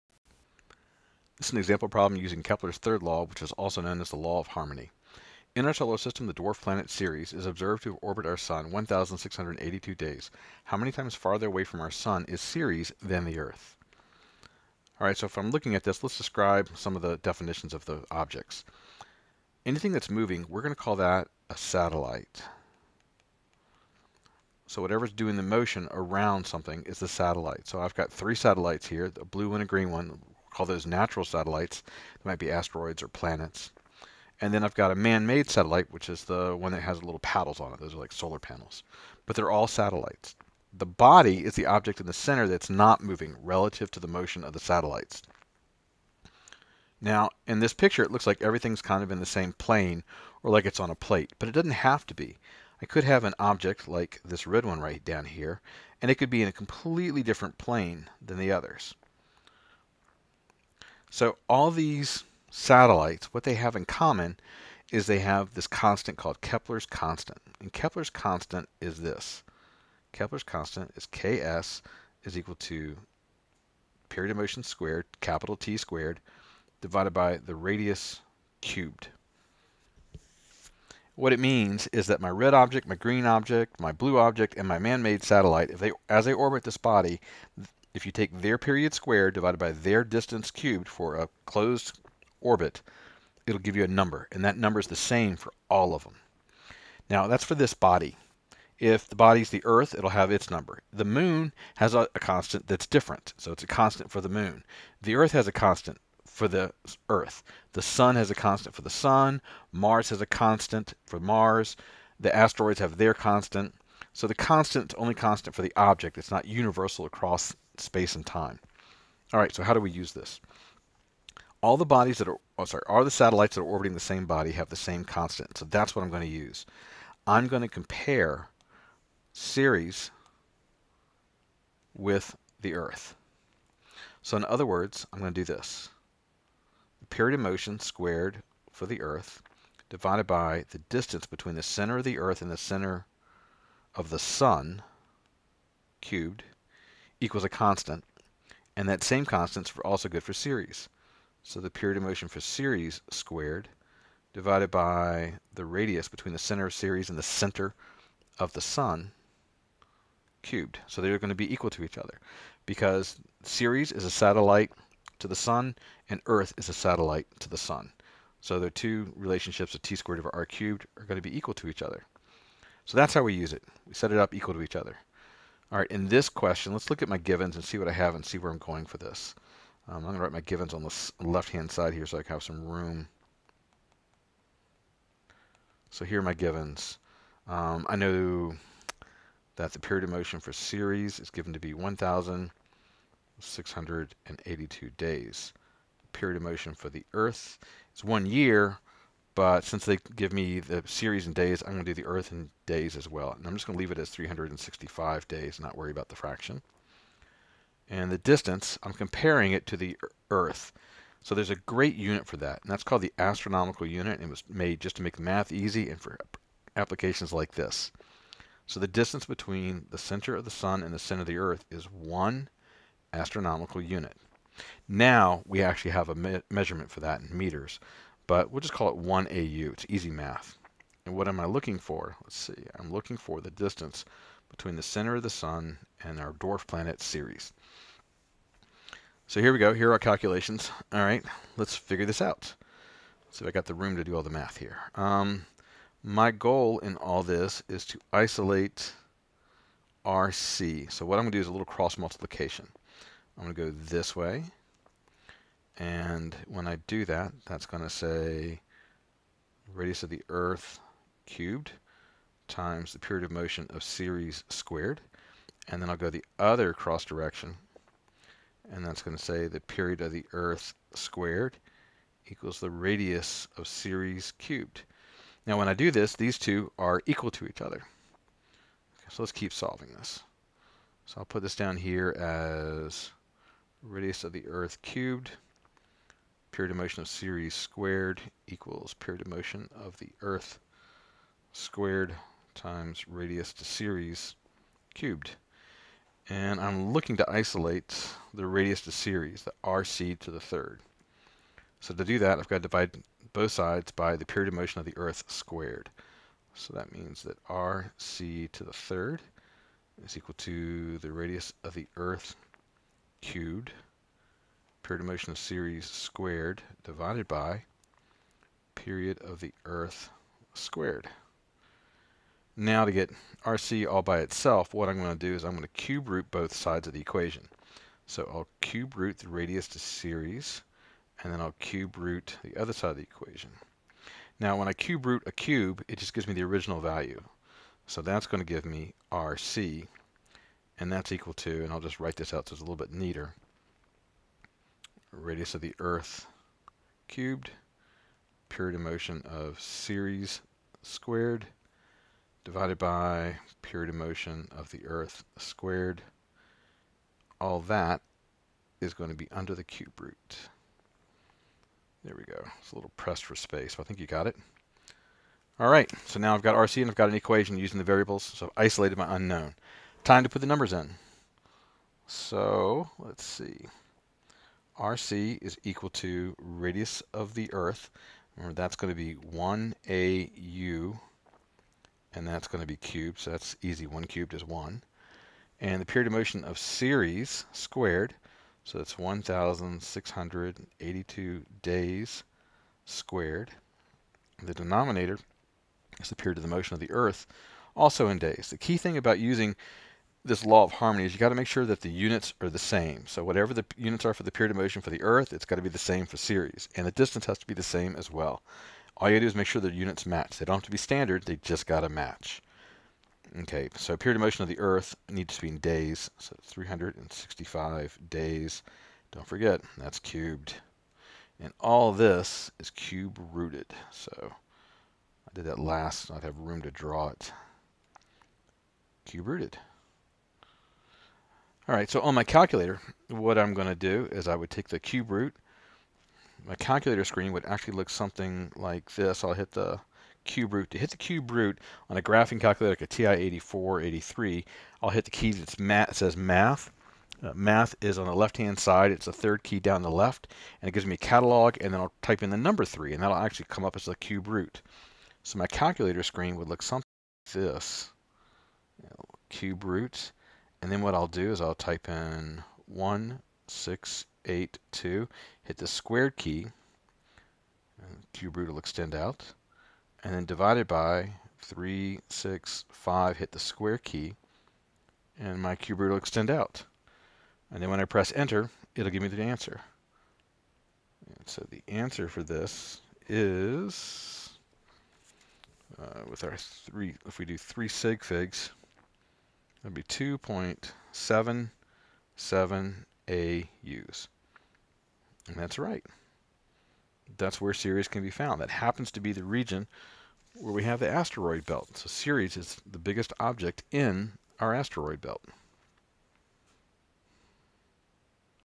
Example Problem for Kepler's 3rd Law
In our solar system the dwarf planet, Ceres, is observed to orbit our sun in 1,682 days. How many times farther away from our sun is Ceres than the Earth?